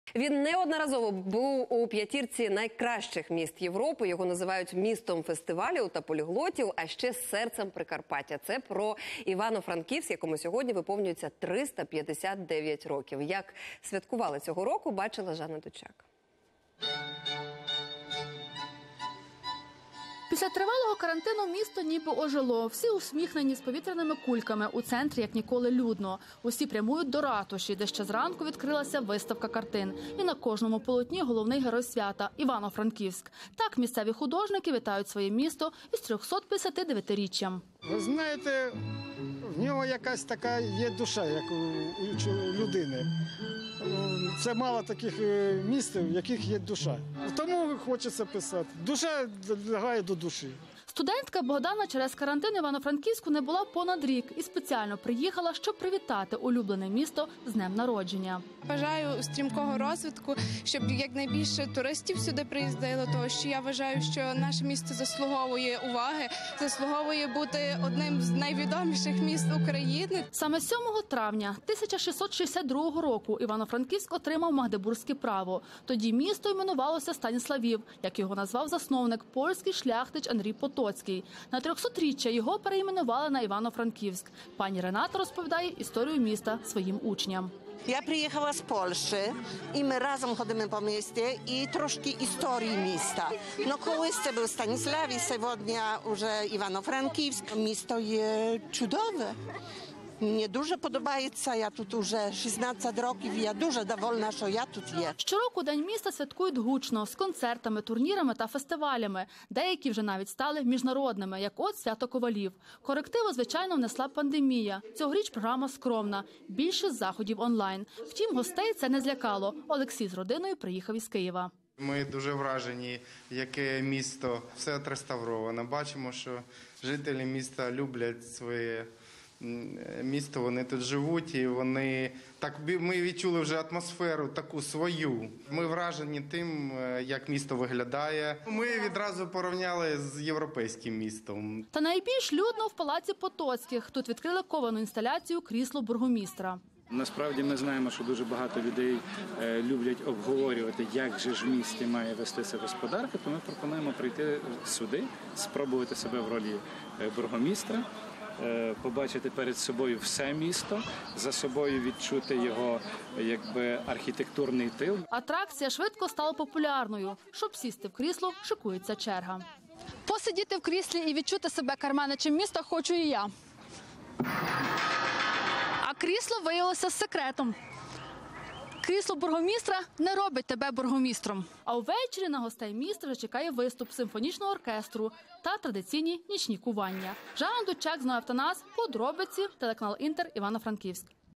Концерти, турніри і фестивалі: Івано-Франківську виповнюється 359 років
Його називають містом фестивалів та поліглотів, а ще серцем Прикарпаття - це все про Івано-Франківськ, якому сьогодні виповнюється 359 років